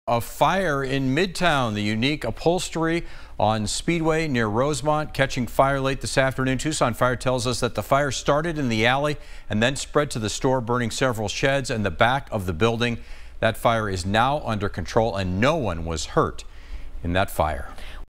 Fire at Unique Upholstery on Speedway
The fire started in the Alley and no one was hurt.